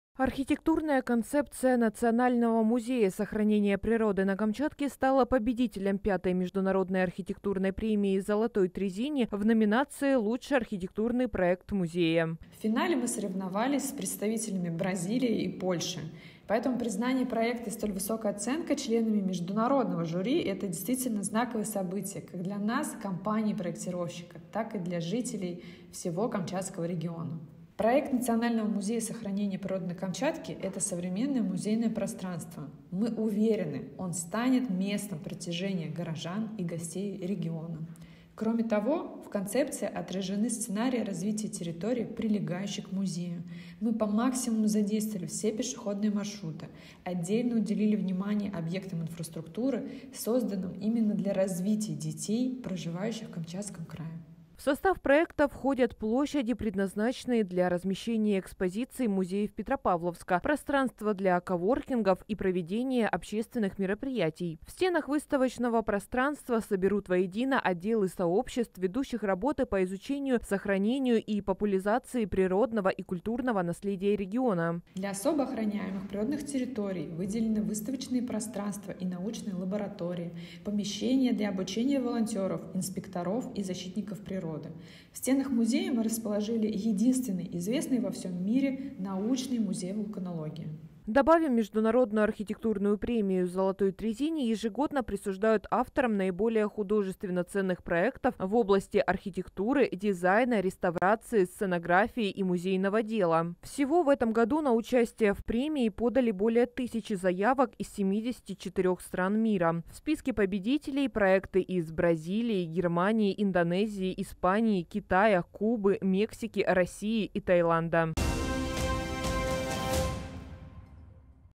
НОВОСТИ КАМЧАТКИ | МАСС МЕДИА | ПОБЕДИТЕЛЬ ПРЕМИИ «ЗОЛОТОЙ ТРЕЗИНИ»
Архитектурная концепция «Национального Музея сохранения природы на Камчатке» стала победителем пятой международной архитектурной премии «Золотой Трезини» в номинации «Лучший архитектурный проект музея».